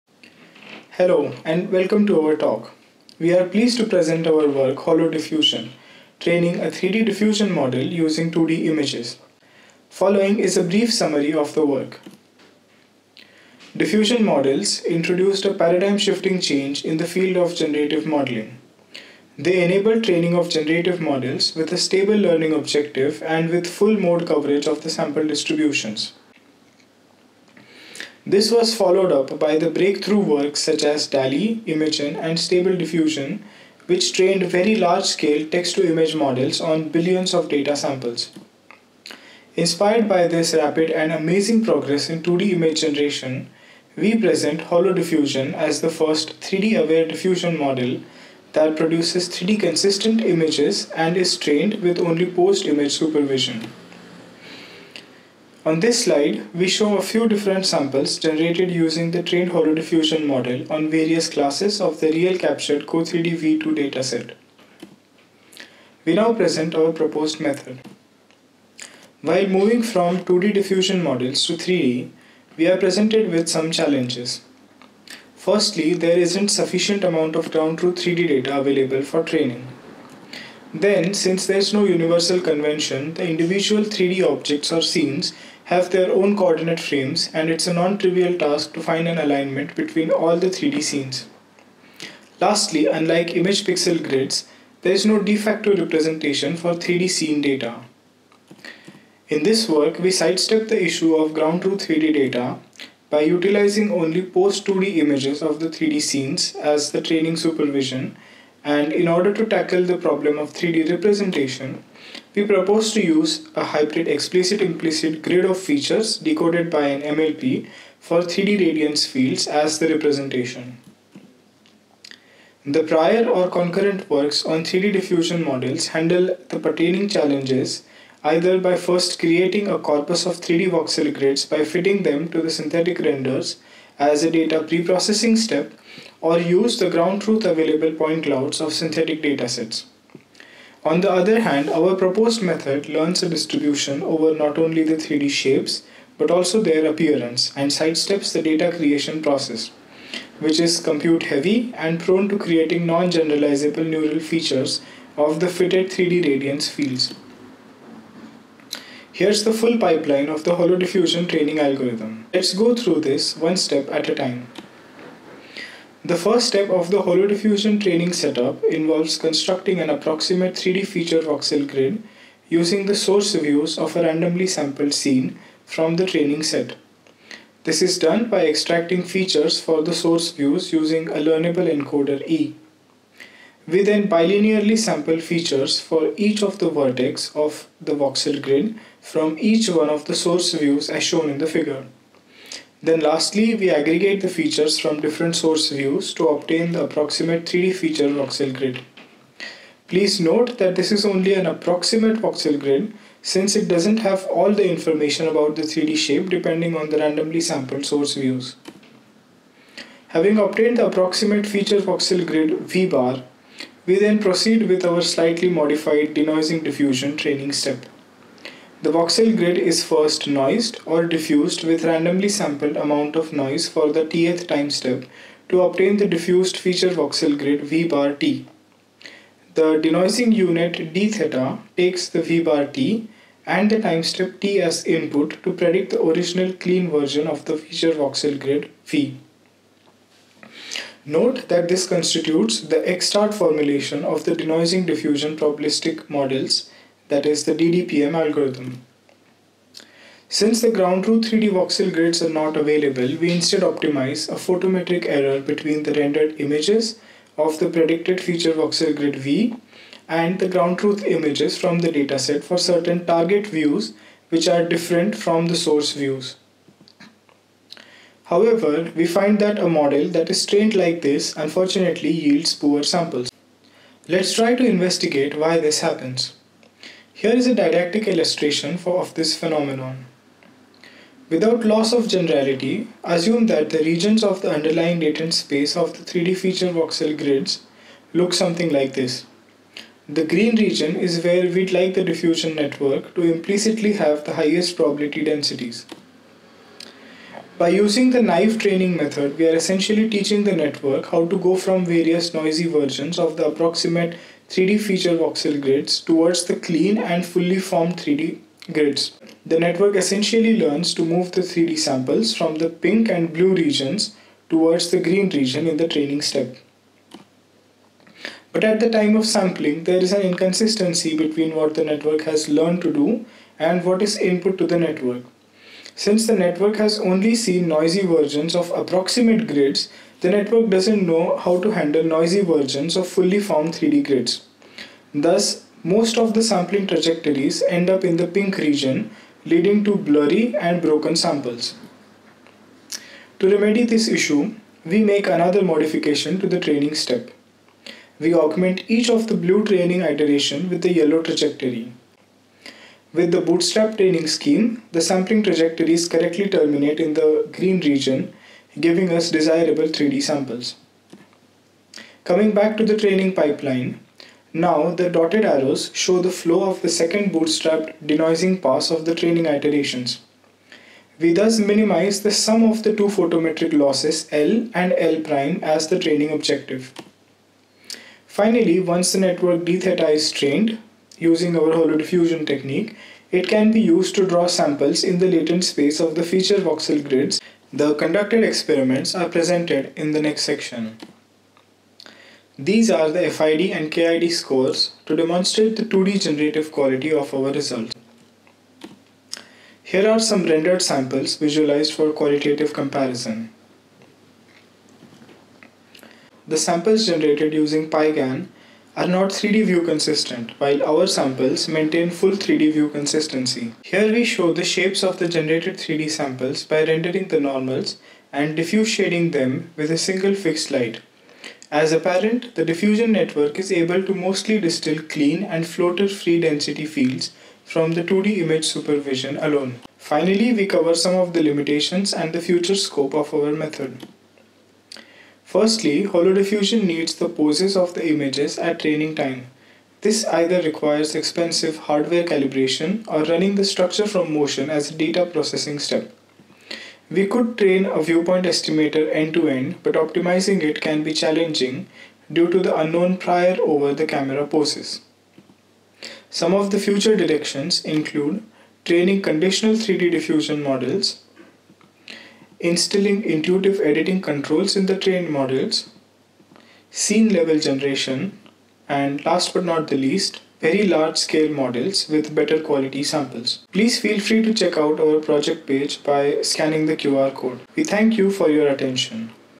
HoloDiffusion: Training a 3D diffusion model using 2D Images
Authors: Animesh Karnewar, Andrea Vedaldi, David Novotny, Niloy Mitra

Diffusion models have emerged as the best approach for generative modelling of 2D images. Part of their success is due to the possibility of training them on millions if not billions of images with a stable learning objective. However, extending these models to 3D remains difficult for two reasons. First, finding a large quantity of 3D training data is much harder than for 2D images. Second, while it is conceptually trivial to extend the models to operate on 3D rather than 2D grids, the associated cubic growth in memory and compute complexity makes this unfeasible. We address the first challenge by introducing a new diffusion setup that can be trained, end to-end, with only posed 2D images for supervision, and the second challenge by proposing an image formation model that decouples model memory from spatial memory. We evaluate our method on real-world data, using the CO3D dataset which has not been used to train 3D generative models before. We show that our diffusion models are scalable, train robustly, and are competitive in terms of sample quality and fidelity to existing approaches for 3D generative modelling.